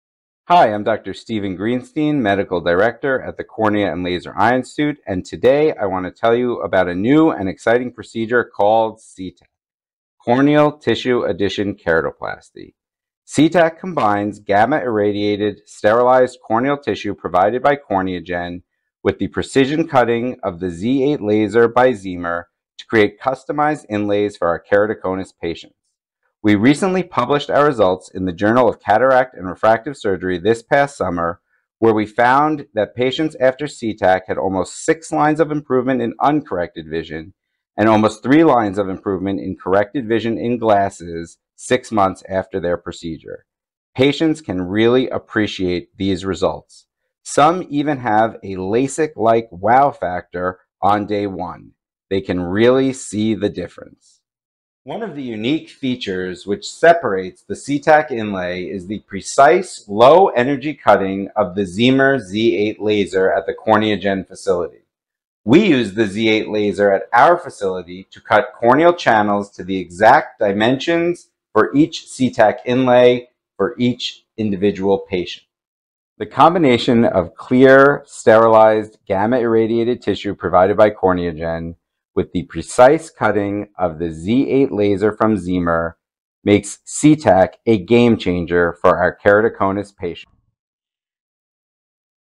CTAK: Corneal Tissue Addition for Keratoplasty
Don't miss out on the opportunity to explore CTAK: Corneal Tissue Addition for Keratoplasty at ASCRS 2024! CTAK is a customized tissue inlay designed from each keratoconus patient’s unique corneal tomography map. Join us at the CorneaGen, Ziemer, and Corza booths for live demonstrations and interactive CTAK wet labs throughout the weekend. Discover how CTAK is revolutionizing keratoconus care and improving patient outcomes!